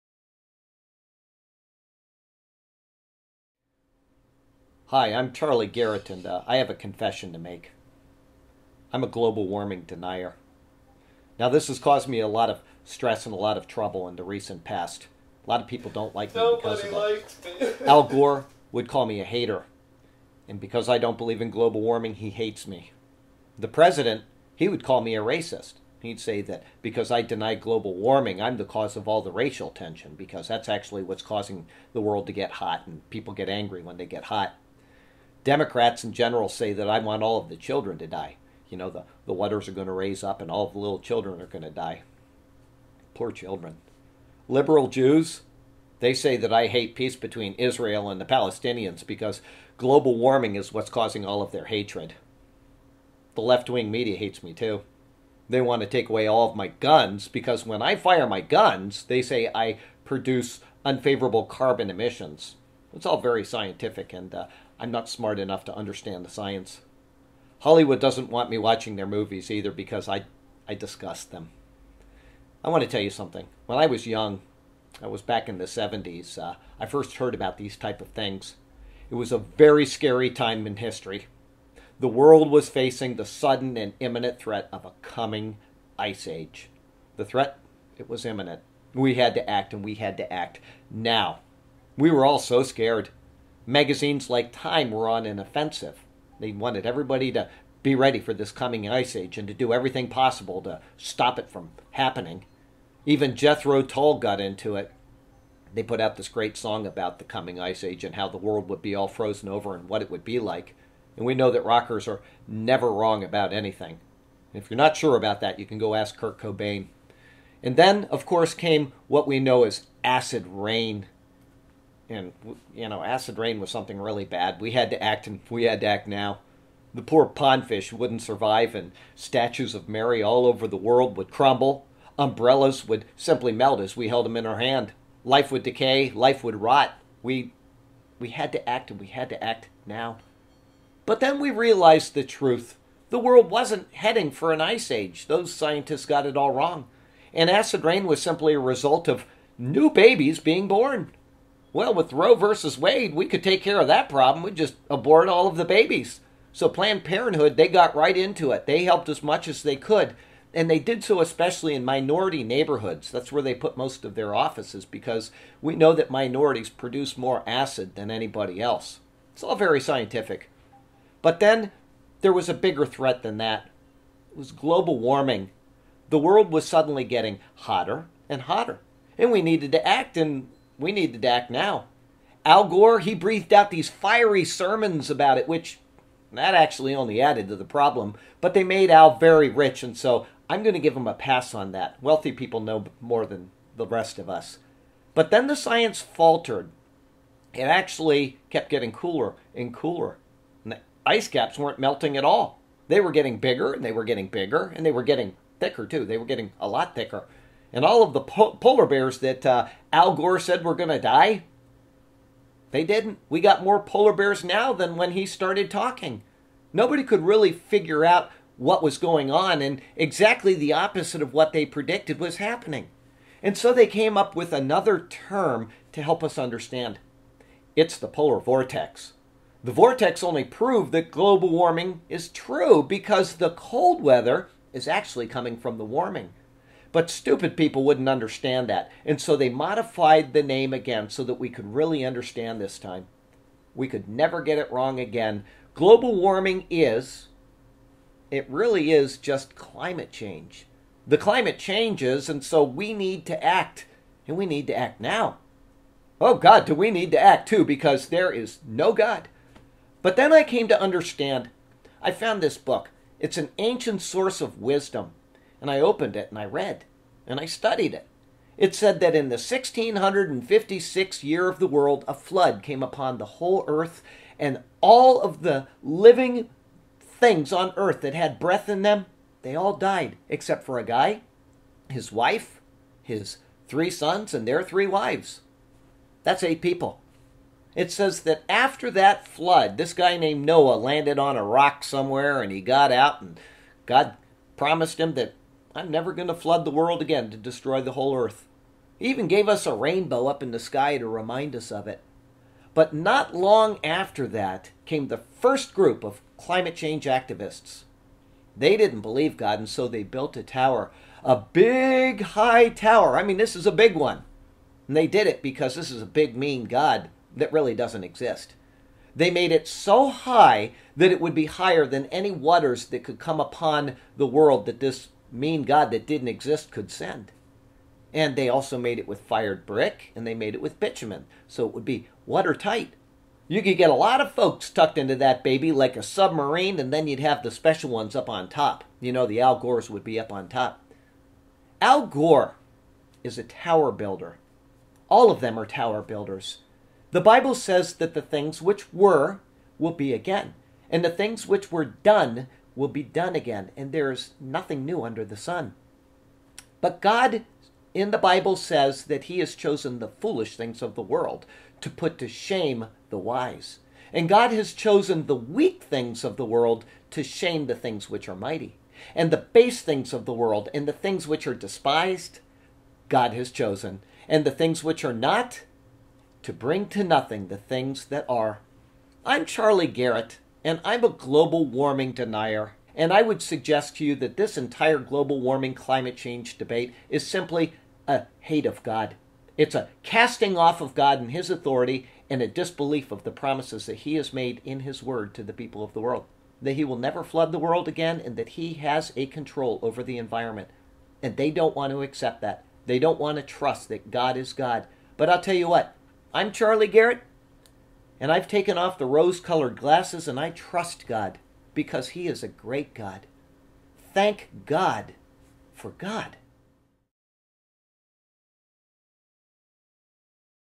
Confessions of a Global Warming Denier - A Story of Trust
An explanation for the bizarre and inane hype about "Global Warming" (which, by the way, is now "Climate Change"). Be a denier by being a believer.